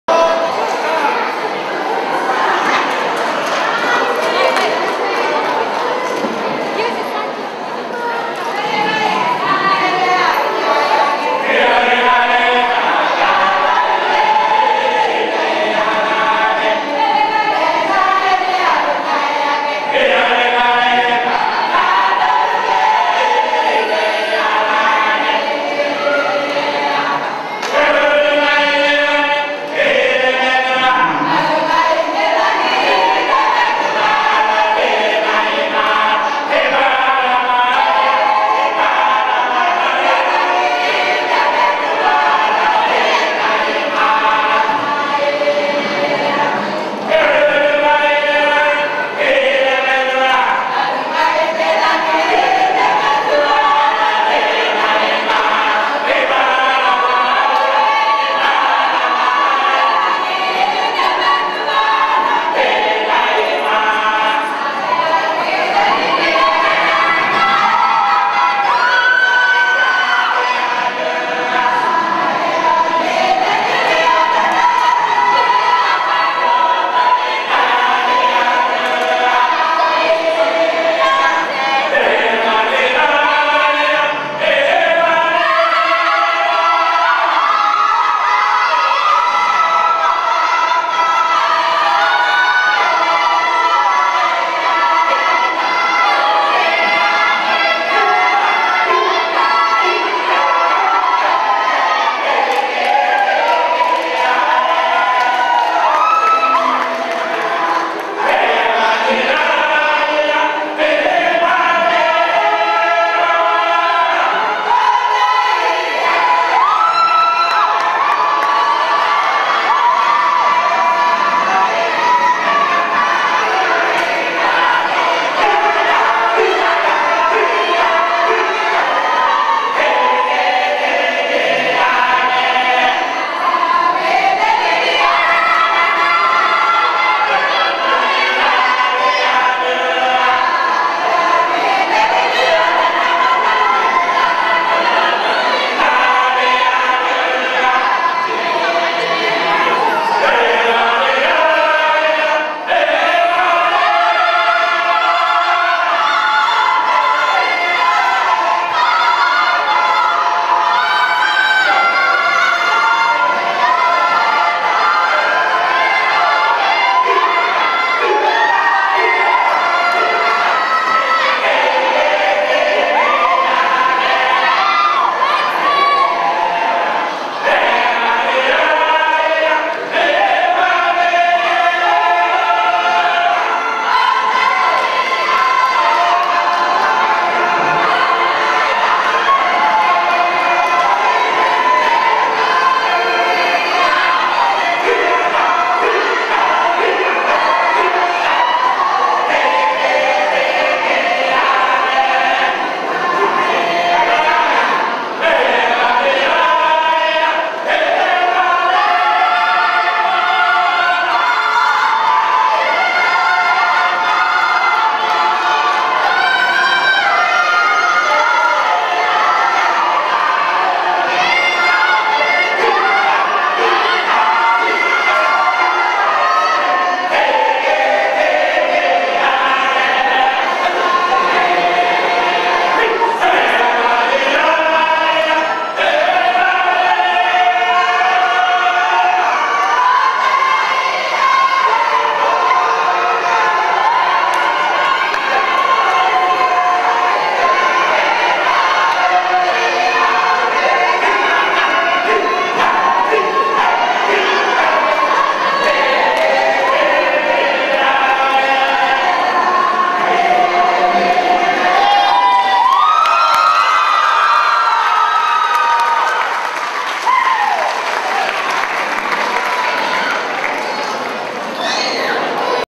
Cook Islands Christian Church Choir, Berala.
Cook Islands Christian Church  Berala @ Alan Walker Pacific Choir Day 26th April 2010. Australia